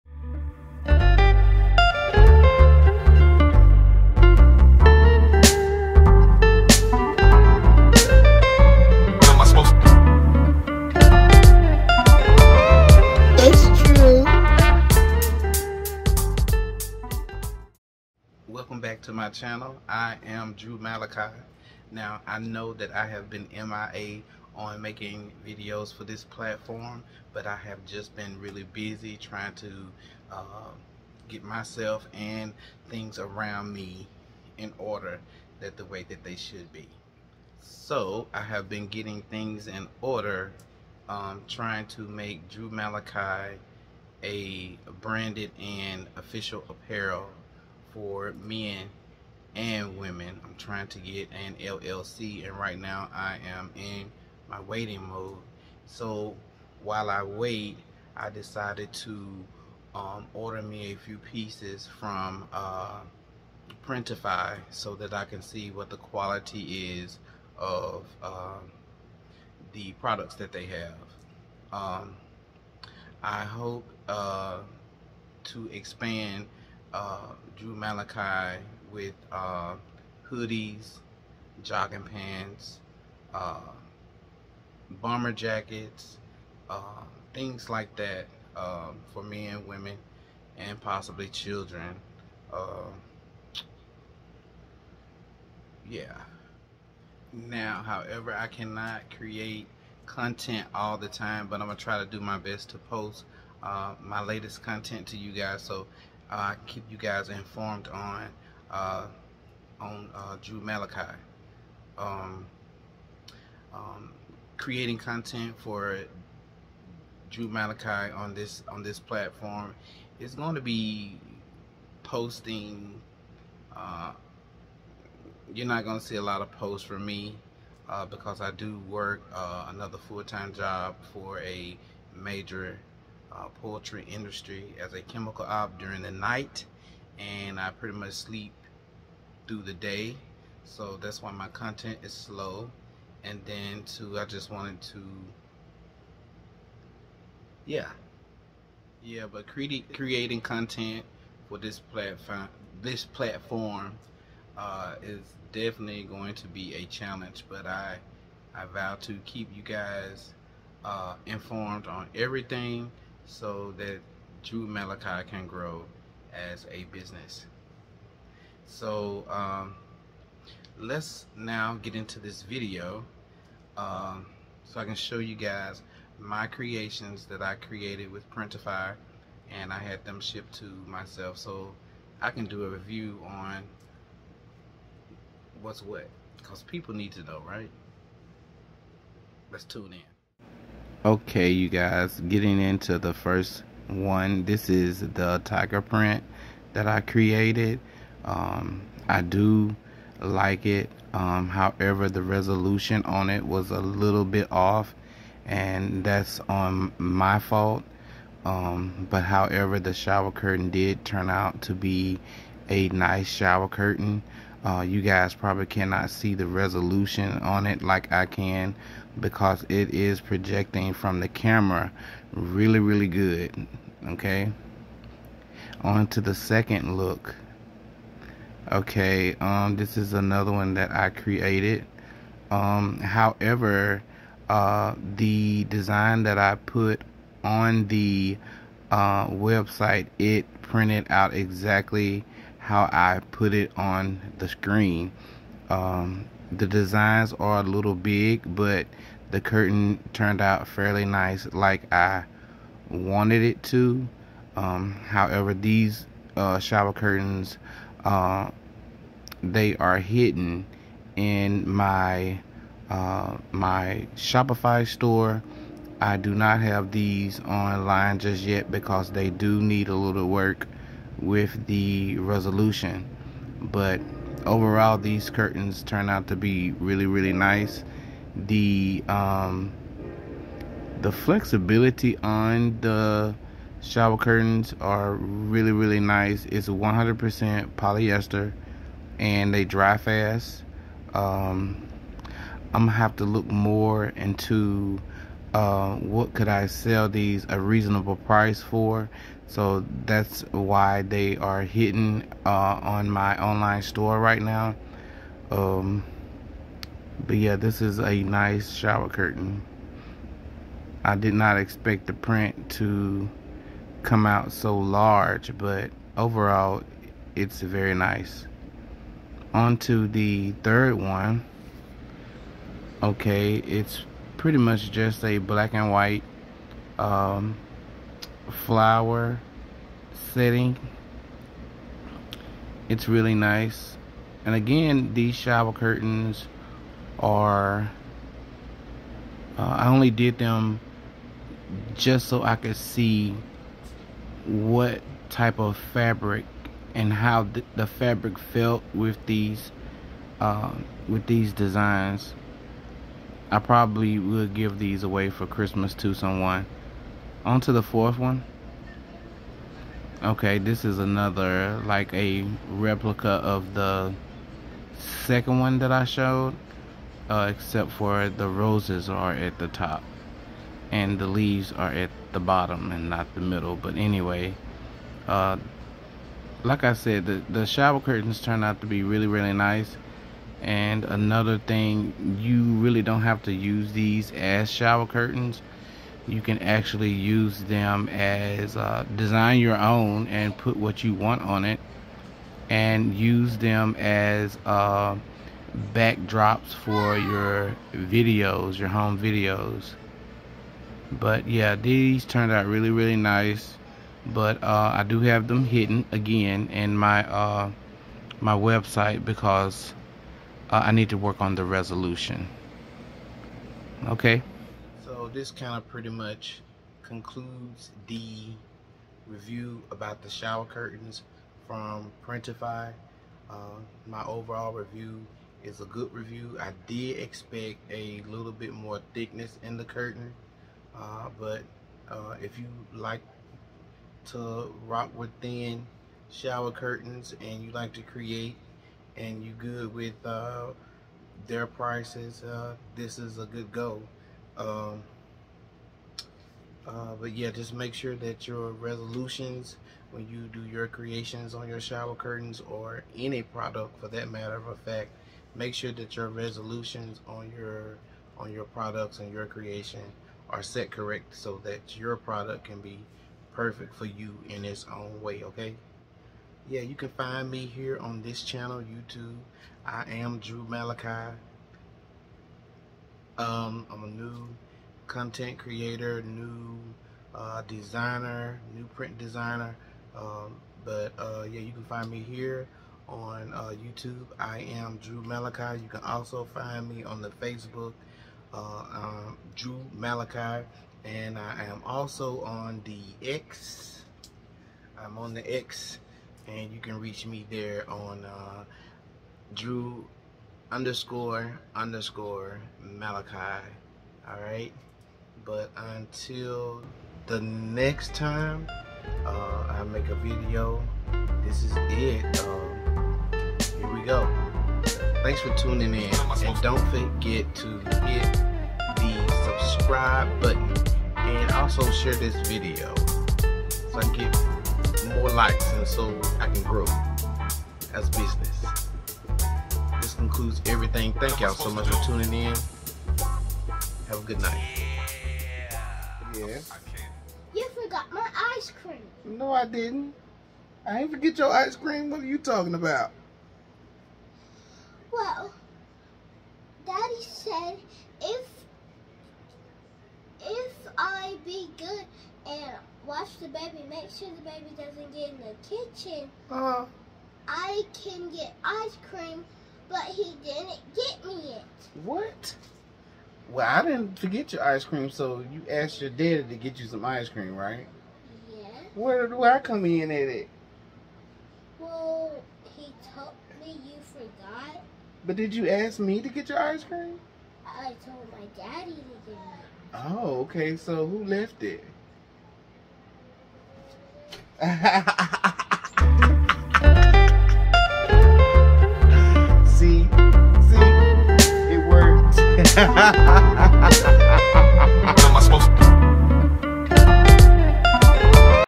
My Shopify Shower Curtain Review-Drew Malachi Business Video Journal
This is my way of reviewing my product that I want to sell in my online store. I want to know the quality and feel of my products. I wanted to know what my customers are receiving when they shop with me. Also that I can set affordable prices.